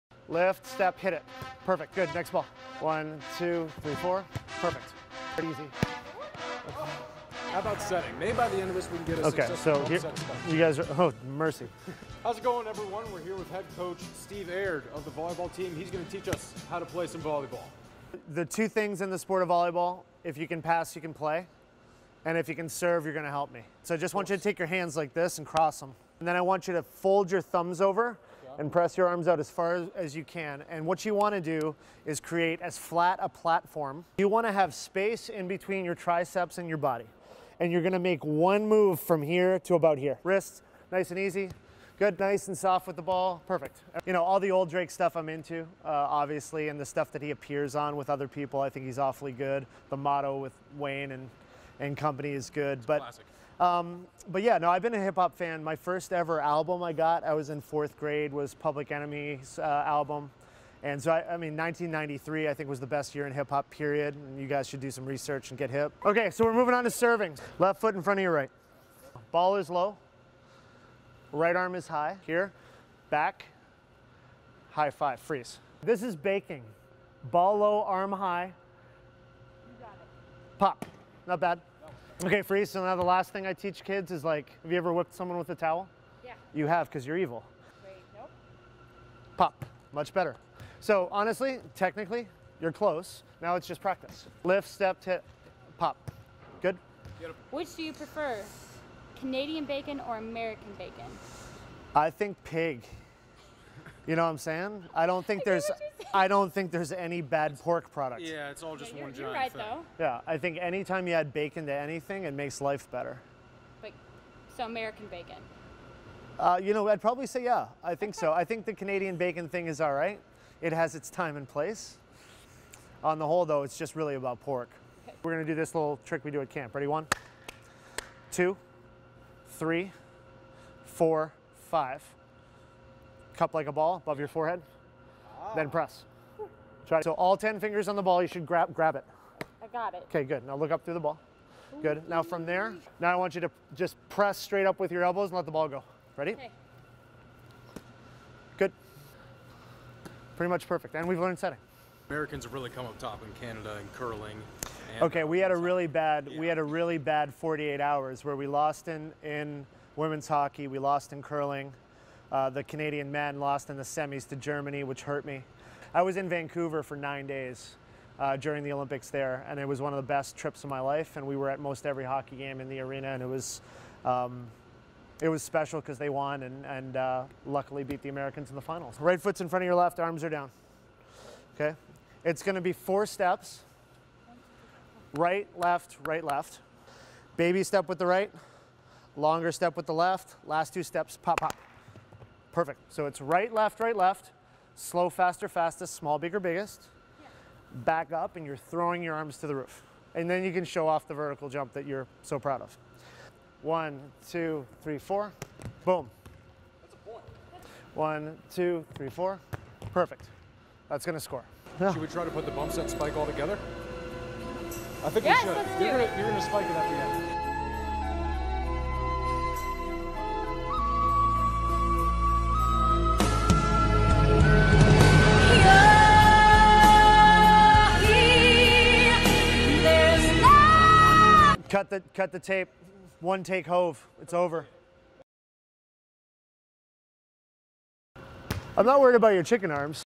How to Play Volleyball - With IU coach Steve Aird
IU Sports Media reporters Griffin Weinberg and Morgan Gard are many things, but volleyball players they are not. Can new IU Volleyball coach Steve Aird help them out? Let's see how it goes.